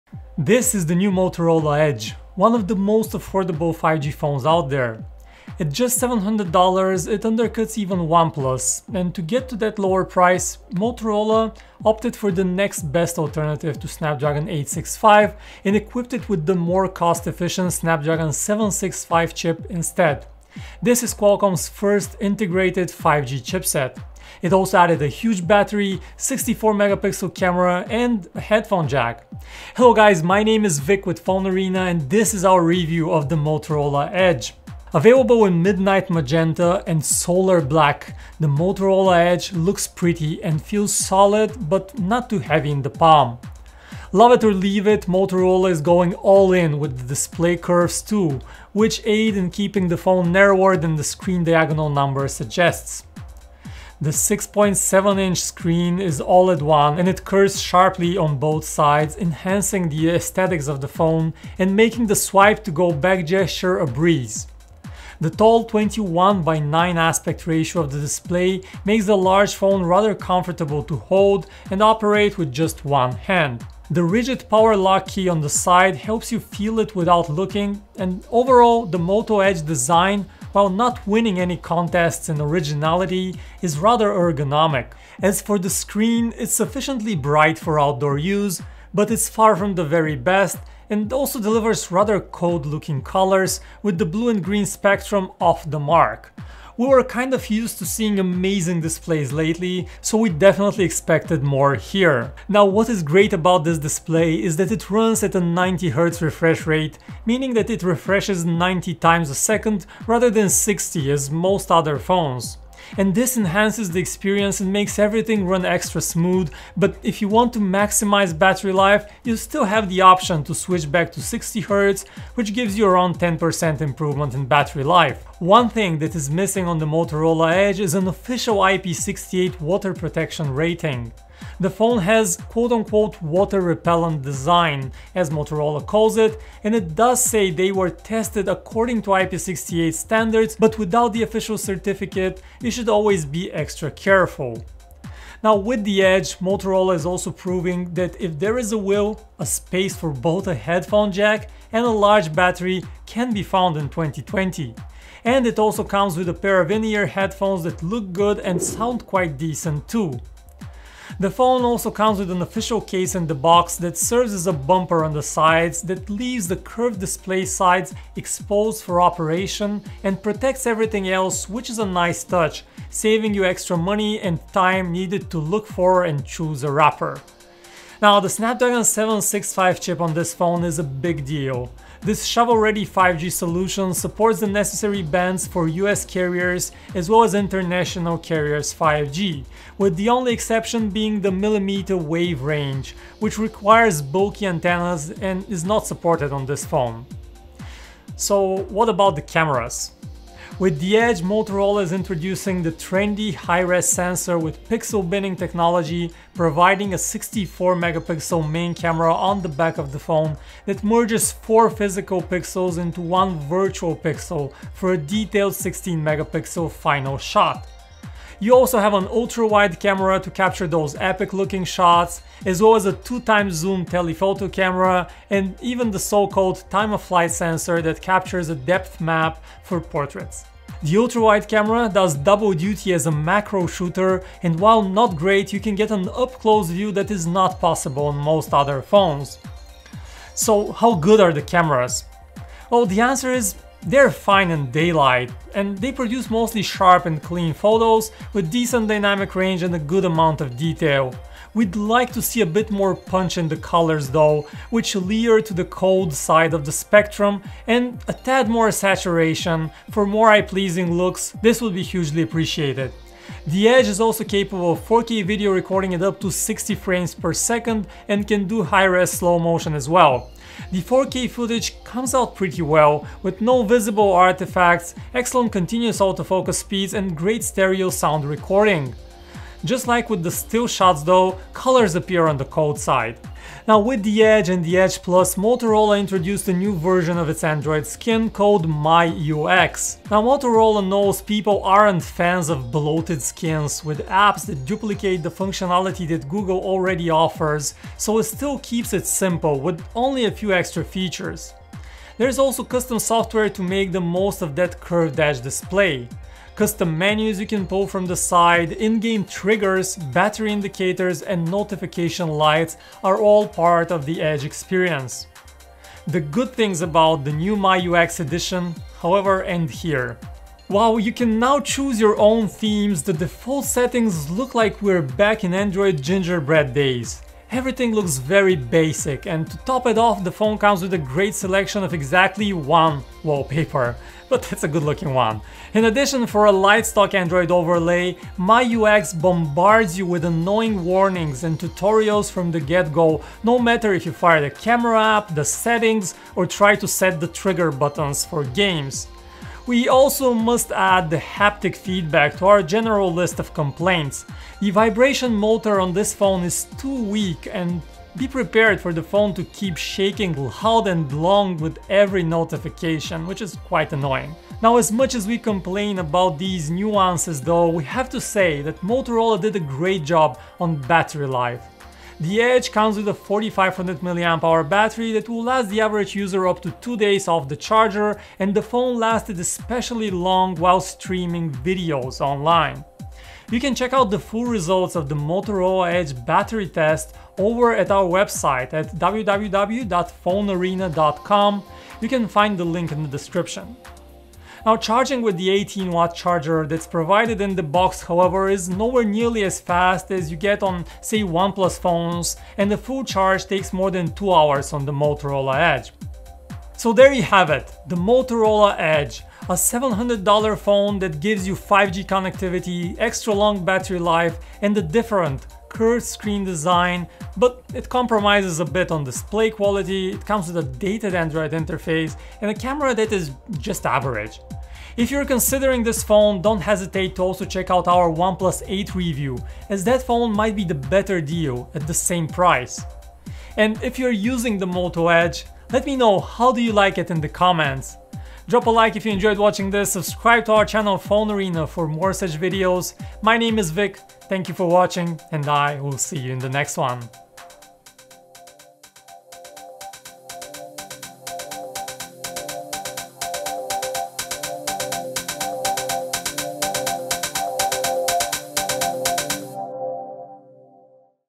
Motorola Edge Review: 5G on a budget
The Motorola Edge is a phone with a screen that wraps around the sides of the phone for a modern look, and it also features 5G connectivity at a price of around $700 with support for both US and international carriers (it supports most bands without mmWave). The Motorola Edge is also powered by the new Snapdragon 765 chip, the same one that is expected to power the Pixel 5 later this fall.